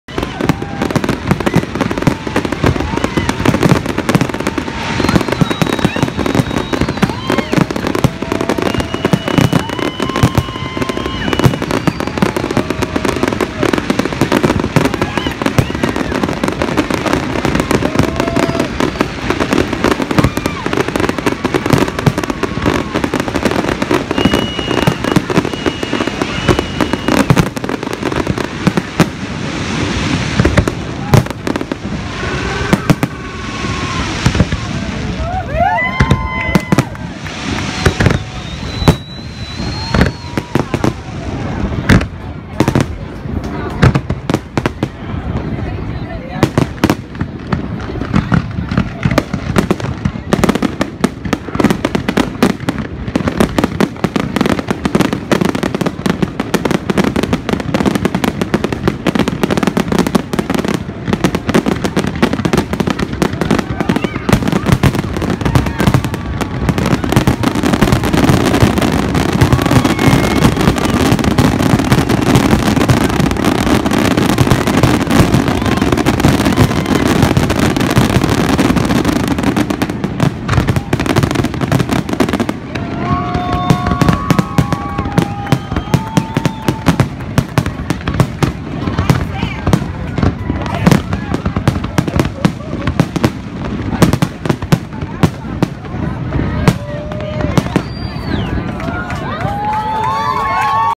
New Year 2016 - Fireworks in West Palm Beach, Florida
Welcoming the Year 2016